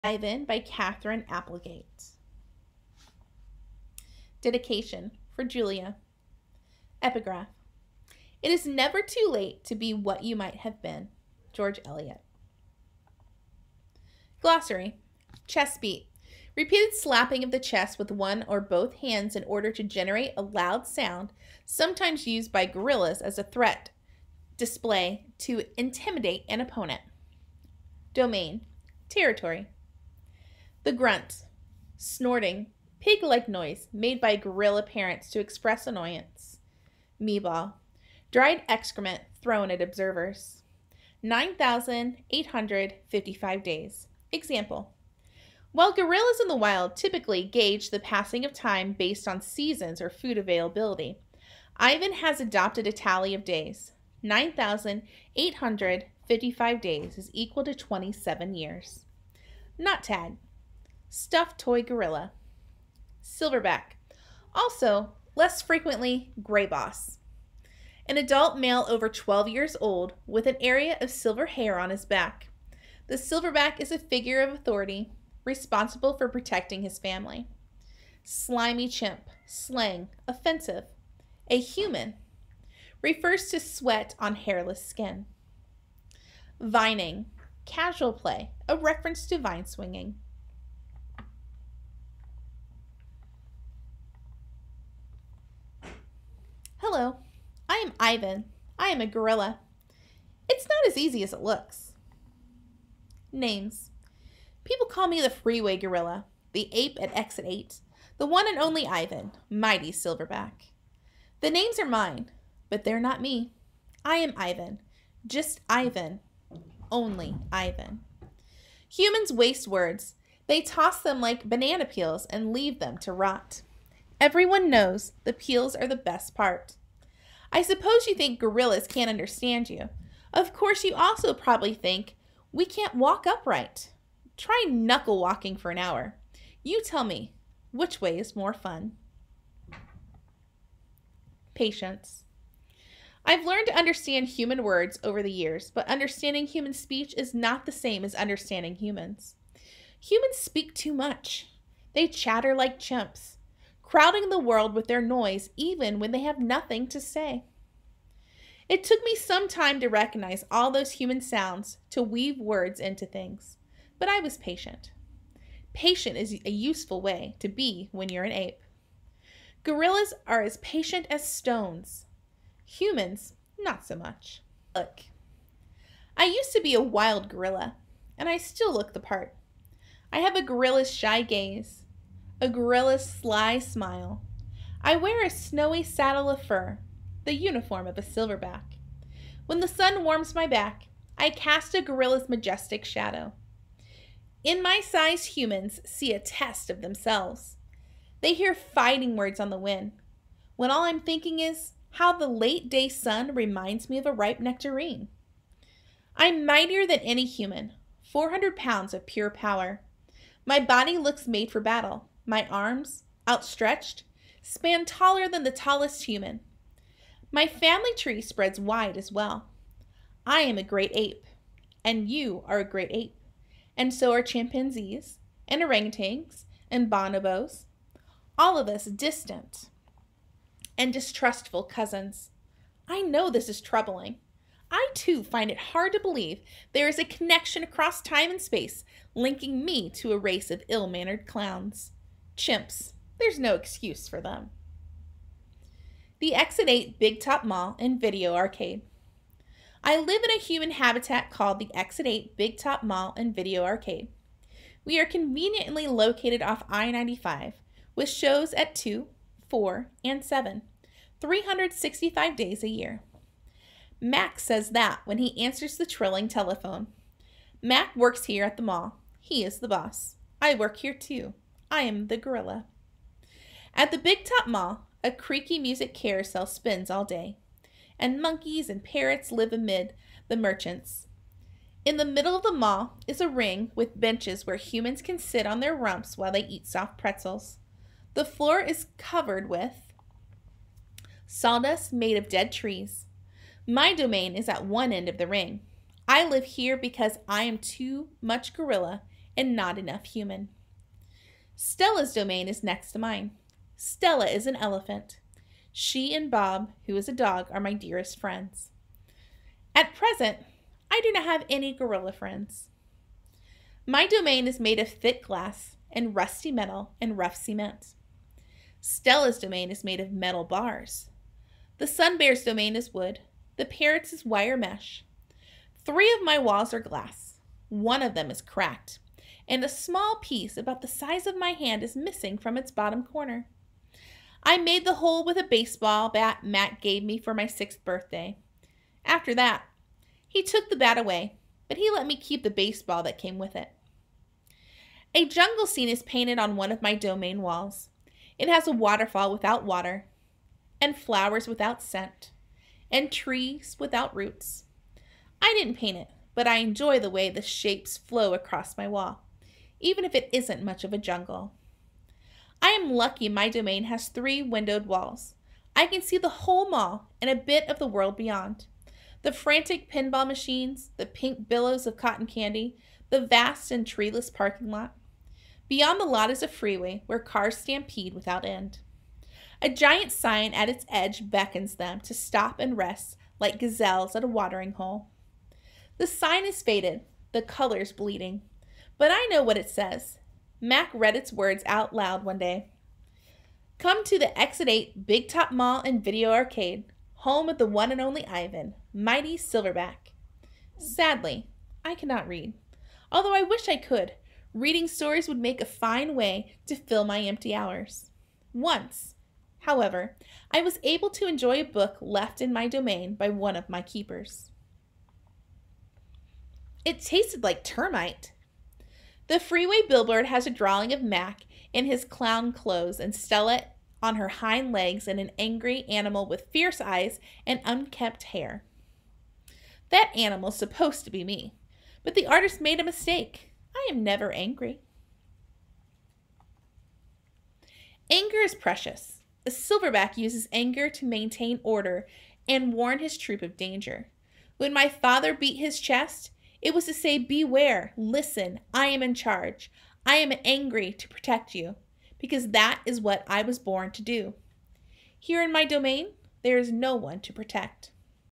Part 1 - The One and Only Ivan
Read Aloud - Epigraph, Glossary, and p. 1-10.
Chapters: hello to the exit 8 Big Top Mall and video arcade